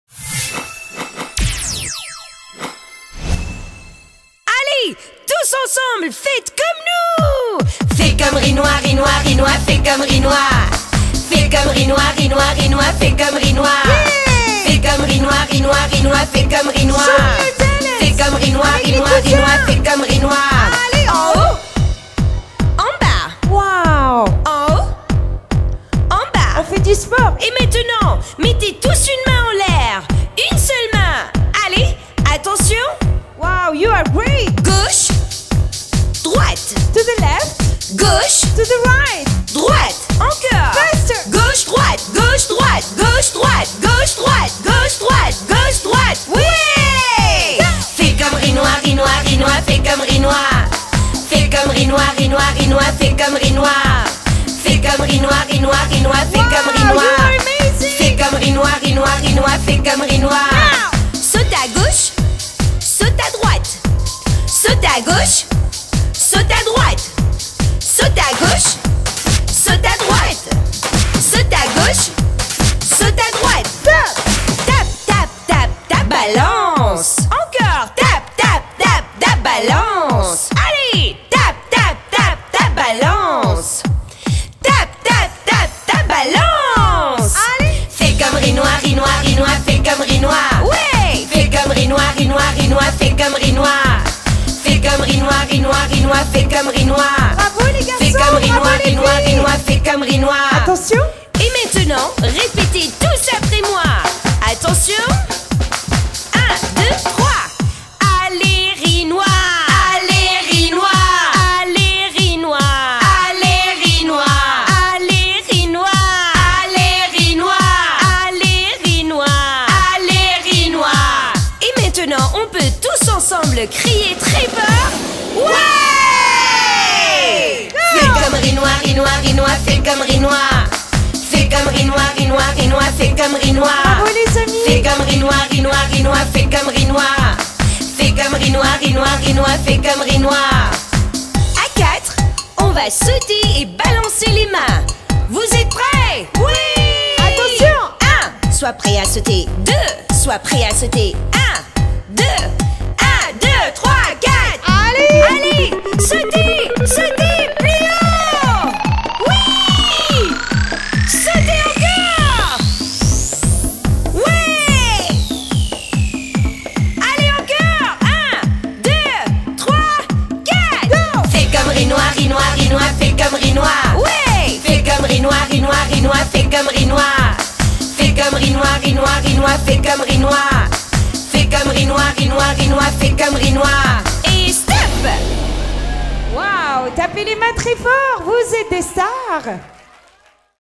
Fais Comme Ghinwa
Our little Workout routine is way more fun than any other routine you've ever tried, just follow our lead and you'll see!!